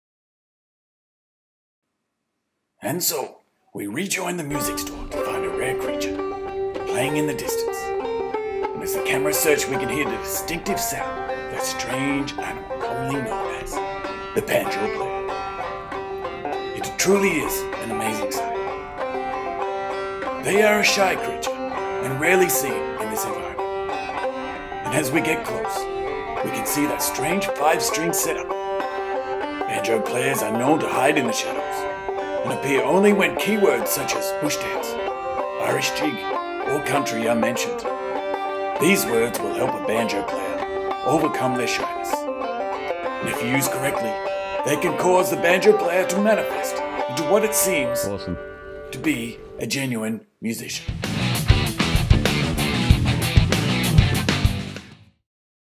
Spying on a closet Banjo player in the shop
At Mooloolaba Music we pride ourselves on researching and practicing as many instruments as we can. Dave was found secretly tooling up on a Deering banjo.
 *This was filmed prior to the virus restrictions.